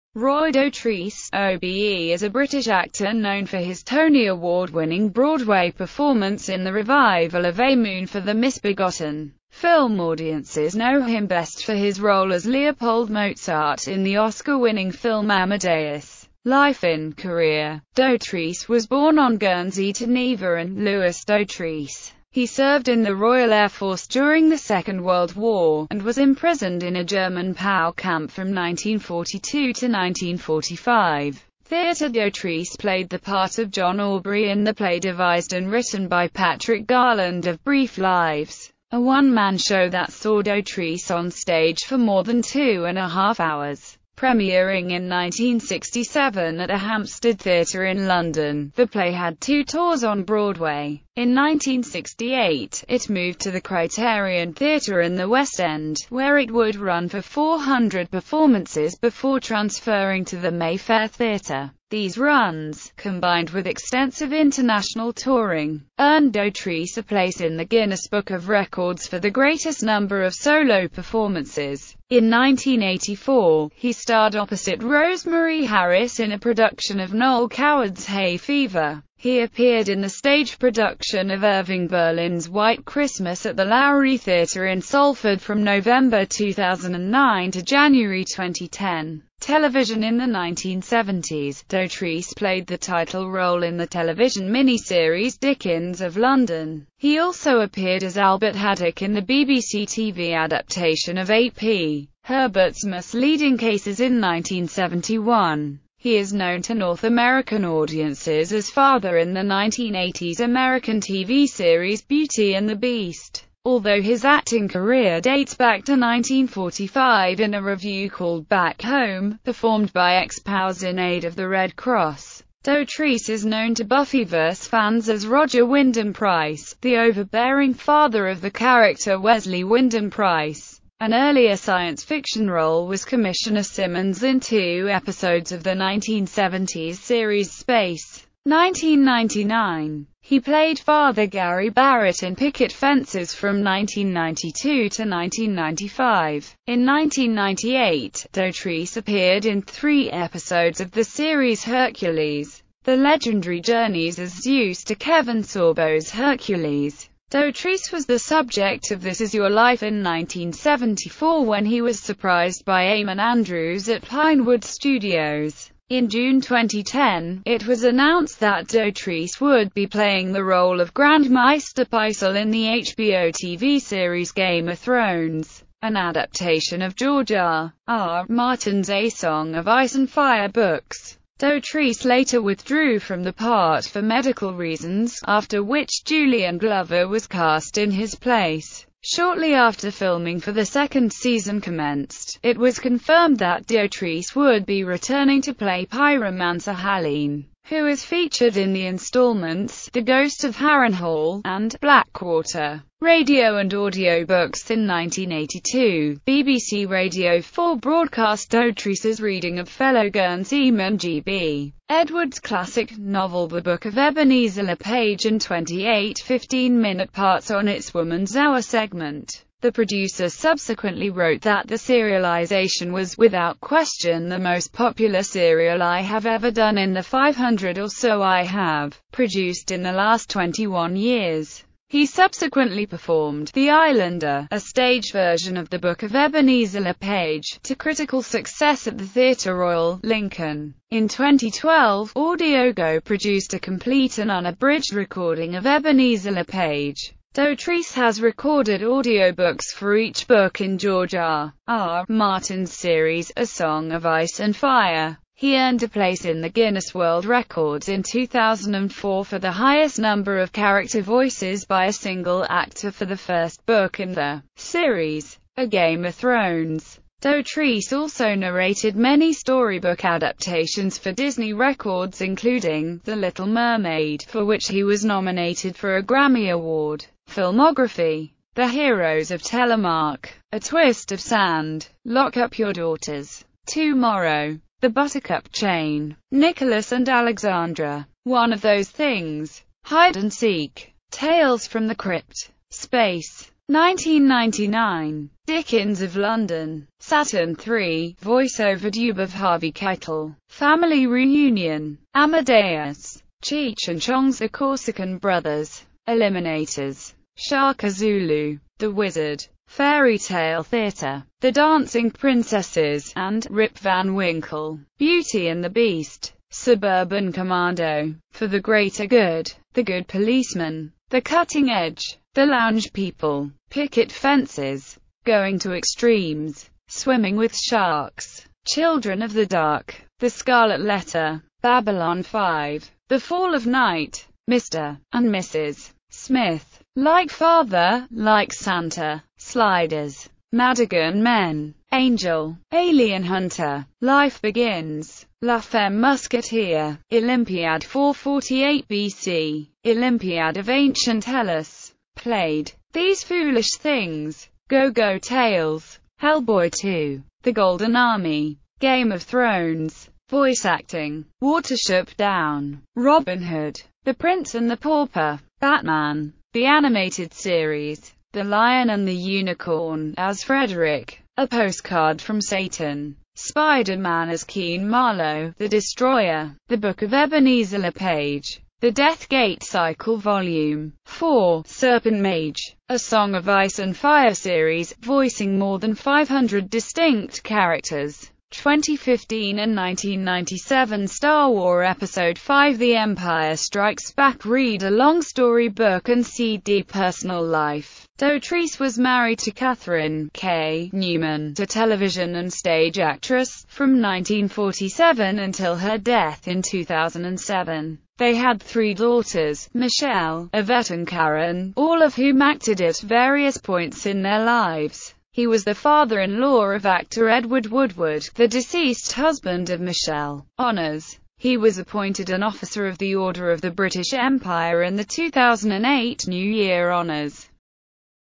Roy Dotrice
Roy Dotrice, OBE (born 26 May 1923) is a British actor known for his Tony Award-winning Broadway performance in the revival of A Moon for the Misbegotten.Film audiences know him best for his role as Leopold Mozart in the Oscar winning film Amadeus.